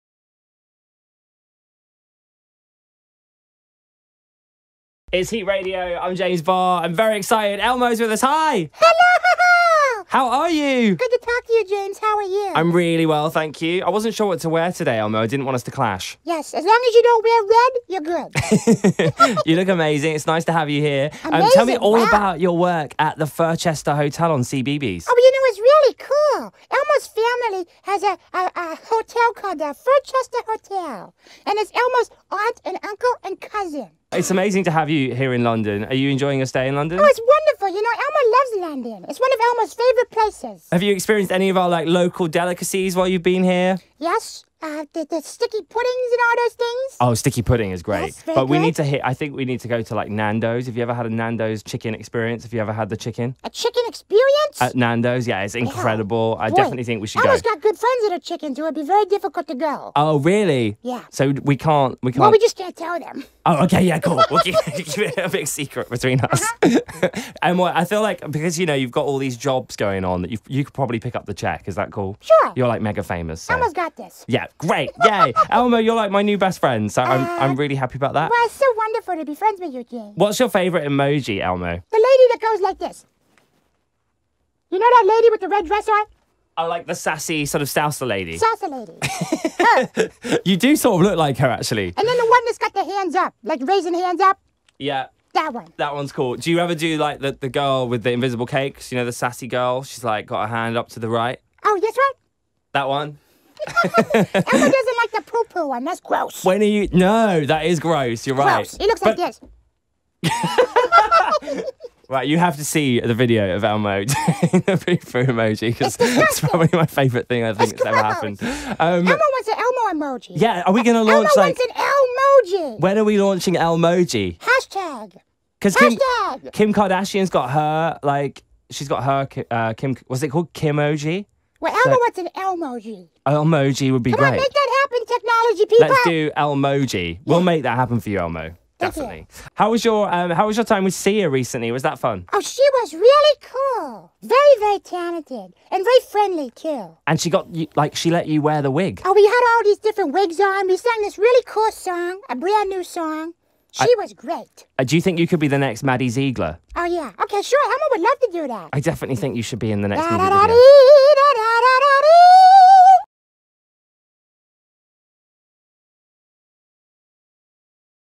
Elmo joins James Barr on heat Radio!
They talk the Furchester Hotel, Nandos, Sia and Elmo having his own emoji!